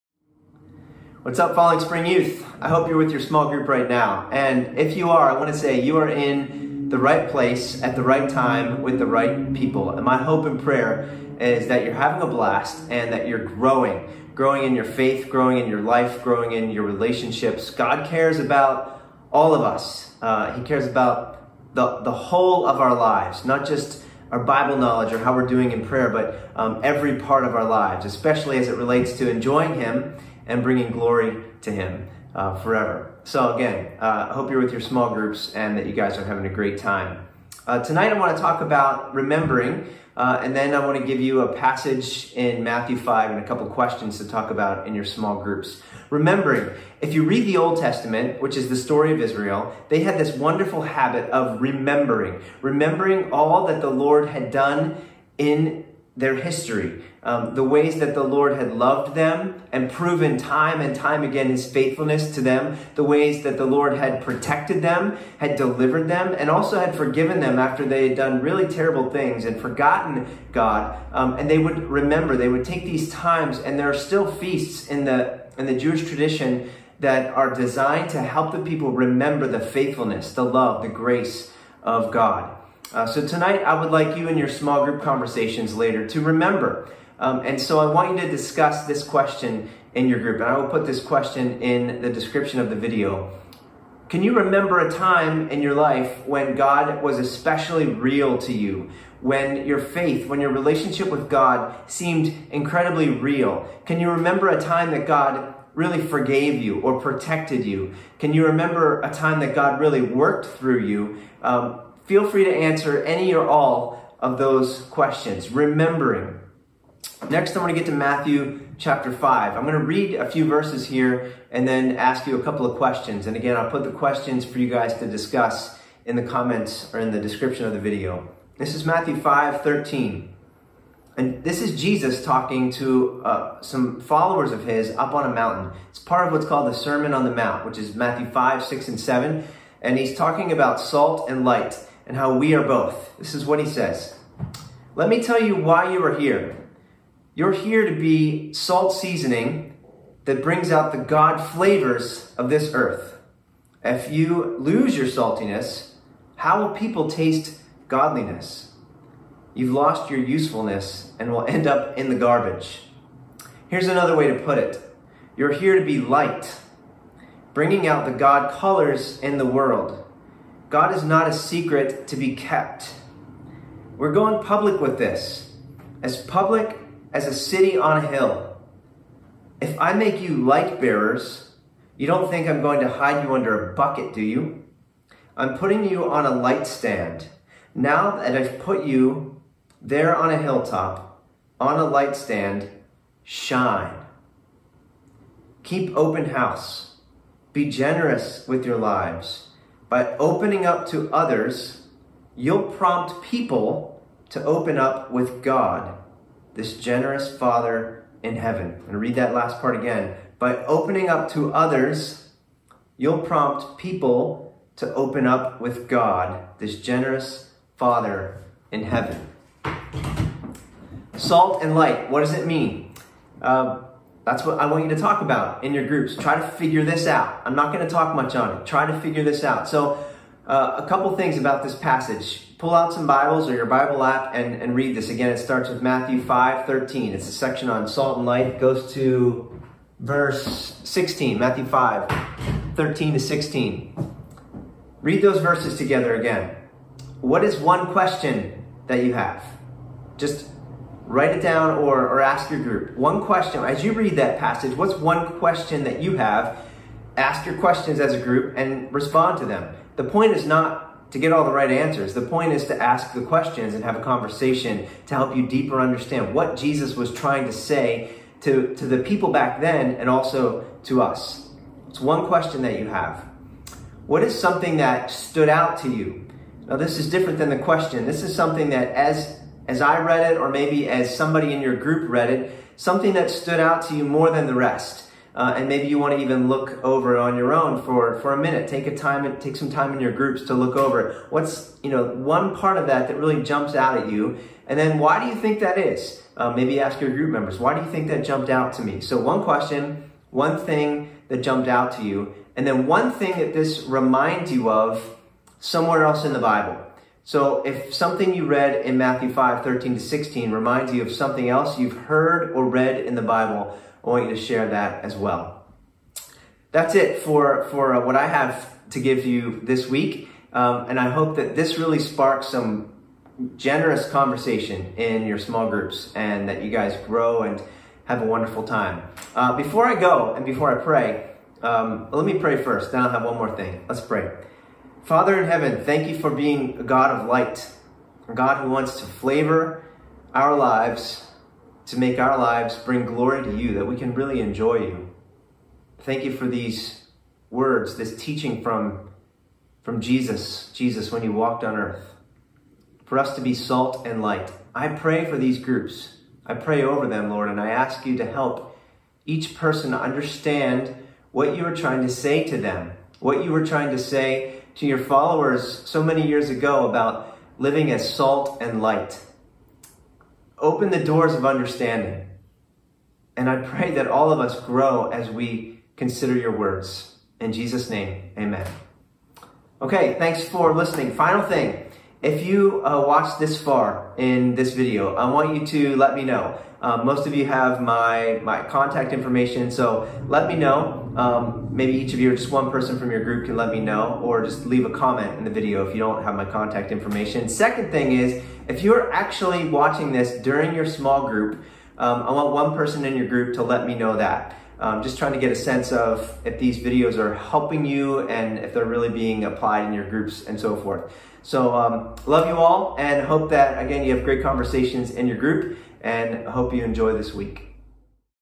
Remembering | Salt | Light
Hello Falling Spring Youth!

Your small group will help you experience LOVE and TRANSFORMATION in Christ. Here are the CONVERSATION QUESTIONS for this week:

What was the best thing that happened to you since we last met?
Remembering. Can you remember a time when God felt especially real to you? When God protected you? When God worked through you to help someone else? When God loved you? Please share these remembrances with your group.
Read Matthew 5:13-16.
What is one question you have about this passage?
What is one thing that stood out in the passage?
Did this passage remind you of any other passage in the Bible?
How can your group pray for you this week?

Please pray for your group members and the other small groups. We love you!